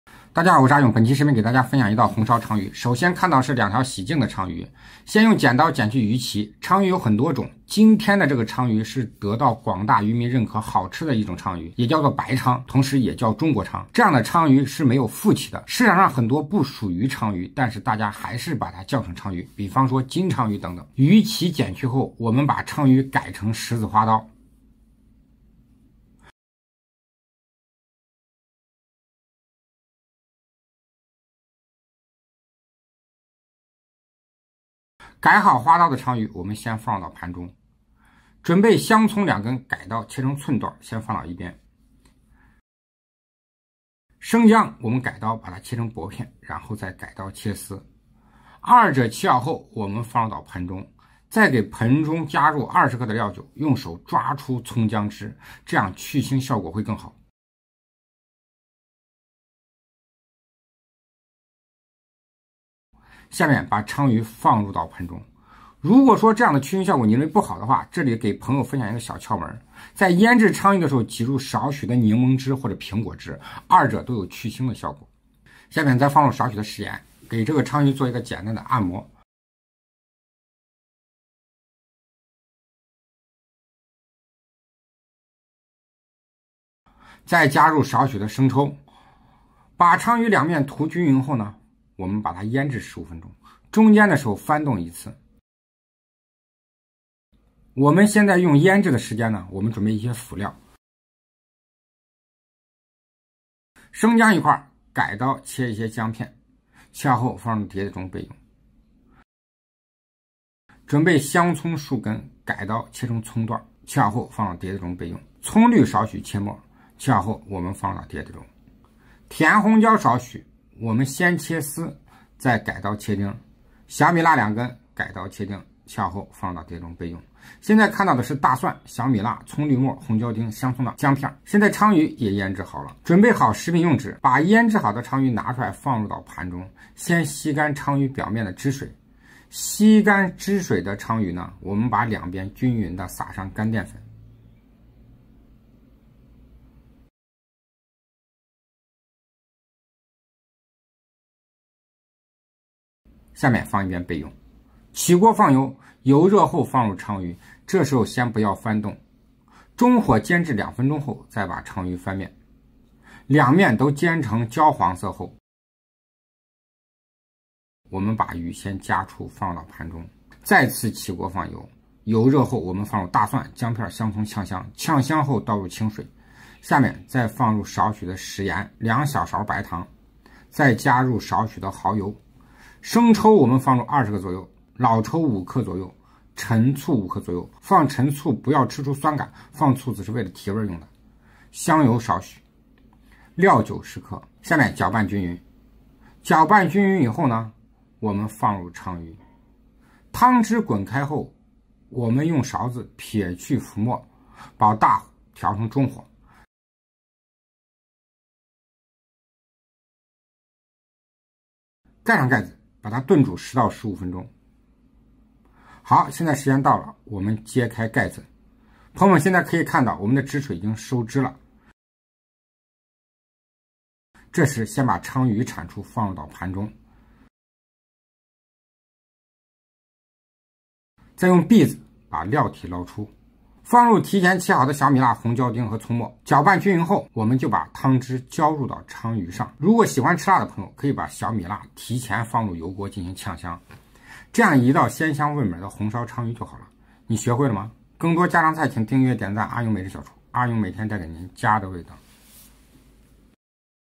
鲳鱼还是红烧更好吃，这样做肉质鲜嫩入味，多汁无腥，下饭又下酒，烧制秘诀全程讲解清楚！
红烧鲳鱼是一道美味佳肴，主要材料有鲳鱼，辅料有大葱、大蒜、辣椒等，口味偏咸鲜。红烧鲳鱼有降低胆固醇的功效，对冠状动脉硬化等心血管疾病有预防作用。